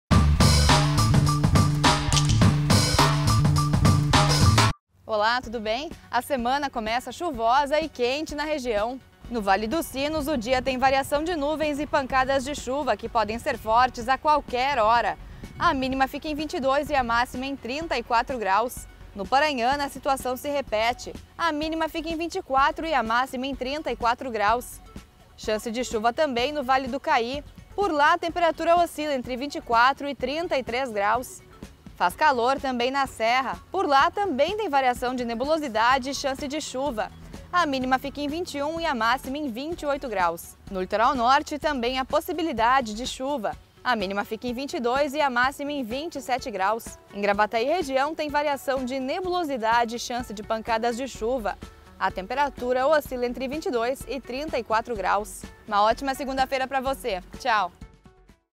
Semana começa com calor e chuva na região
Máxima passa dos 30ºC nesta segunda-feira